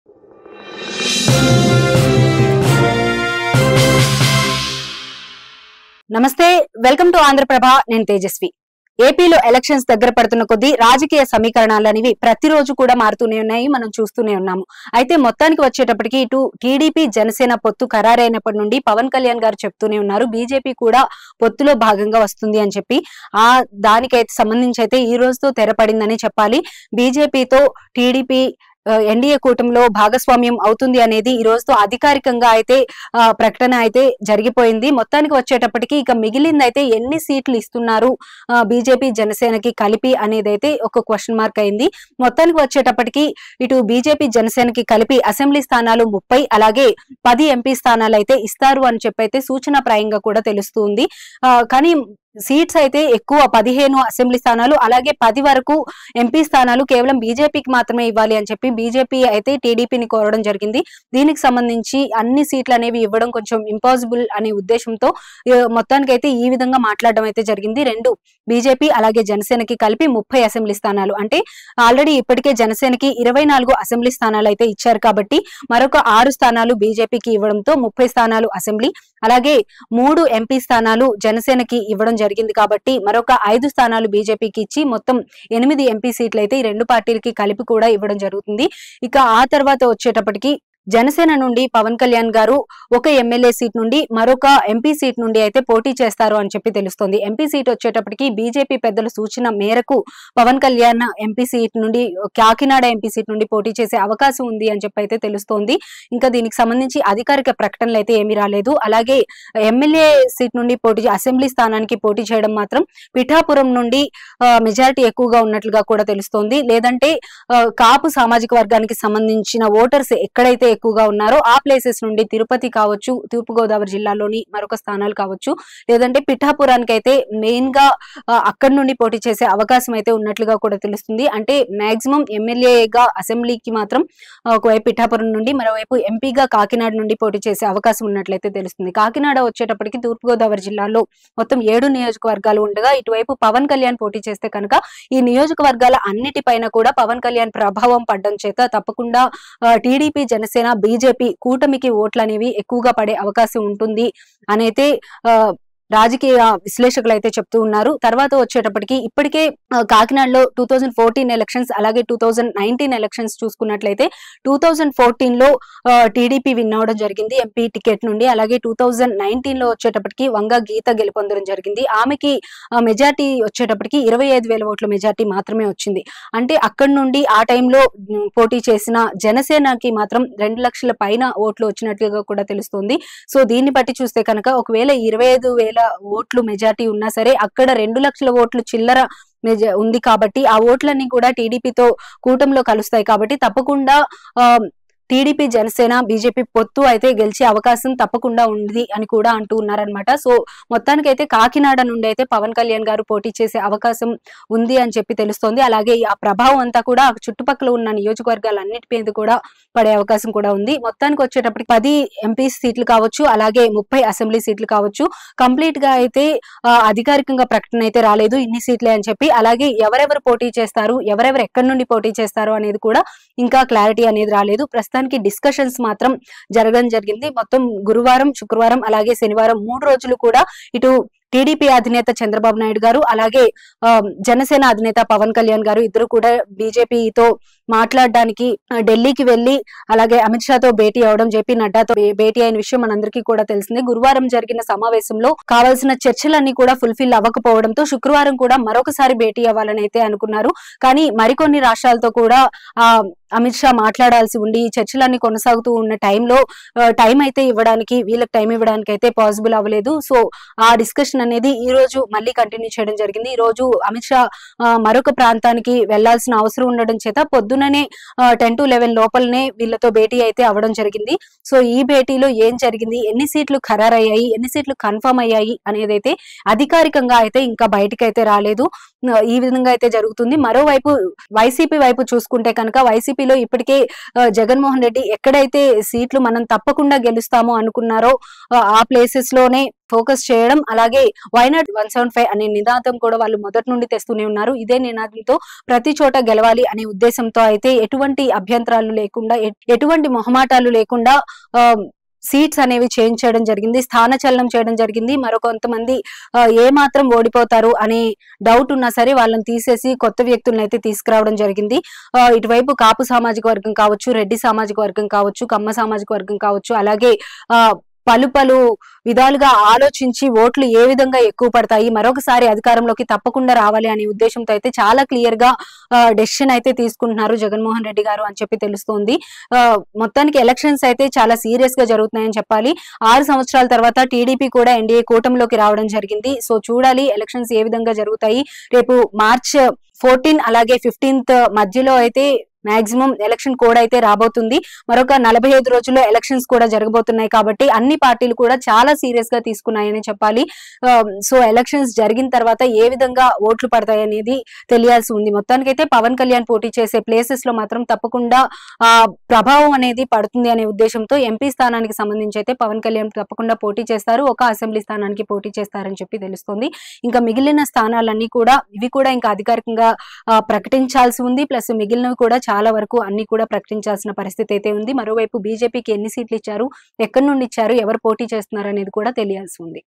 పొత్తు కుదిరింది, సీట్ల వ్యవహారం మొదలైంది..!! | AP Politics | TDP and Janasena Alliance with BJP
పొత్తు కుదిరింది, సీట్ల వ్యవహారం మొదలైంది..!! | AP Politics | TDP and Janasena Alliance with BJP | Andhraprabha News